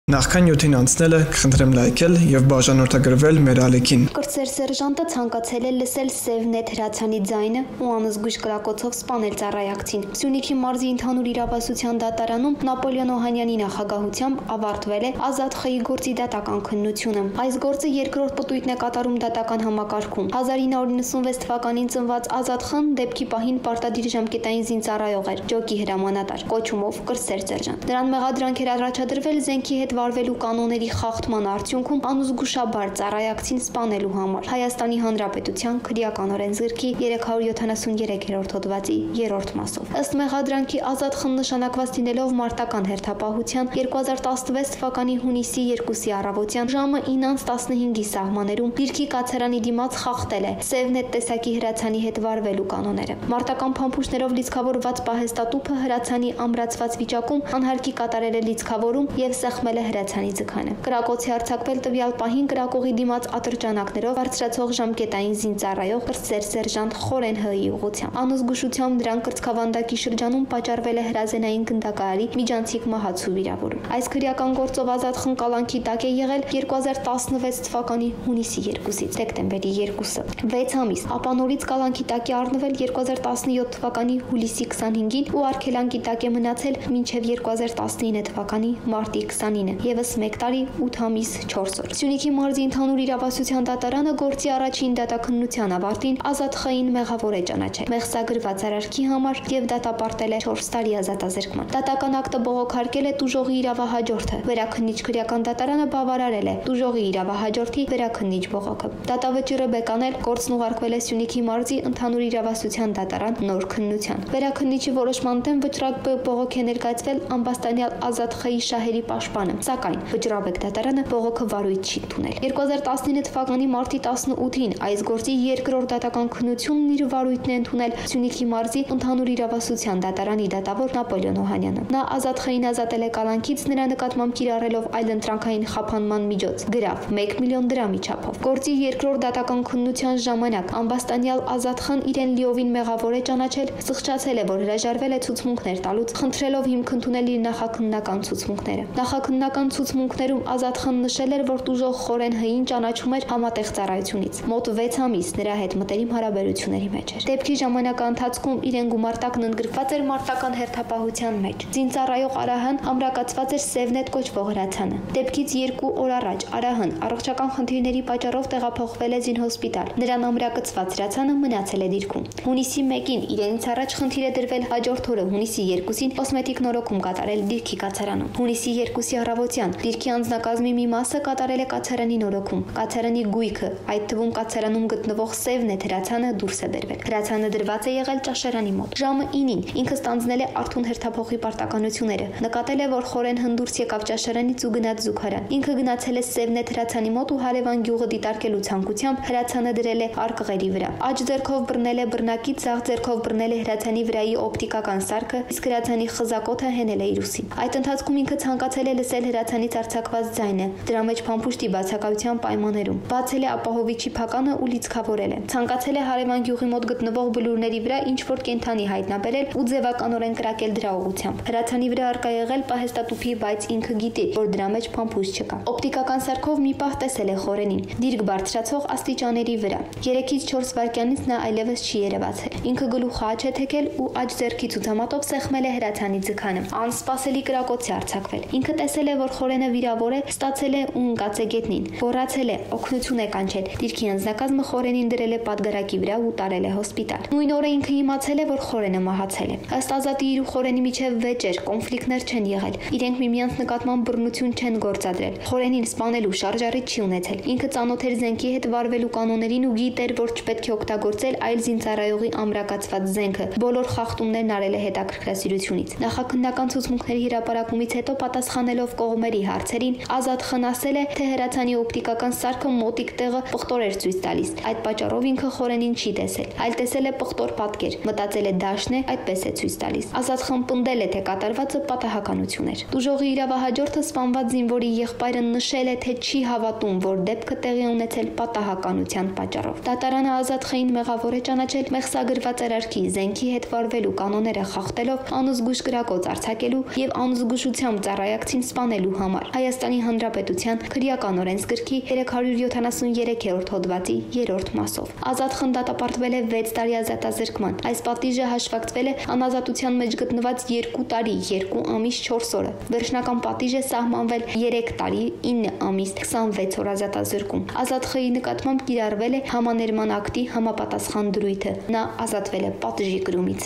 Կրտսեր սերժանտը ցանկացել է լսել «Սև նետ» հրացանի ձայնը ու անզգույշ կրակոցով սպանել ծառայակցին
Սյունիքի մարզի ընդհանուր իրավասության դատարանում Նապոլեոն Օհանյանի նախագահությամբ ավարտվել է Ազատ Խ.-ի գործի դատական քննությունը:

Այս գործը երկրորդ պտույտն է կատարում դատական համակարգում:

1996 թվականին ծնված Ազատ Խ.-ն դեպքի պահին պարտադիր ժամկետային զինծառայող էր, ջոկի հրամանատար, կոչումով՝ կրտսեր սերժանտ: Նրան մեղադրանք էր առաջադրվել զենքի հետ վարվելու կանոնների խախտման արդյունքում անզգուշությամբ ծառայակցին սպանելու համար՝ ՀՀ քրեական օրենսգրքի 373 հոդվածի 3-րդ մասով: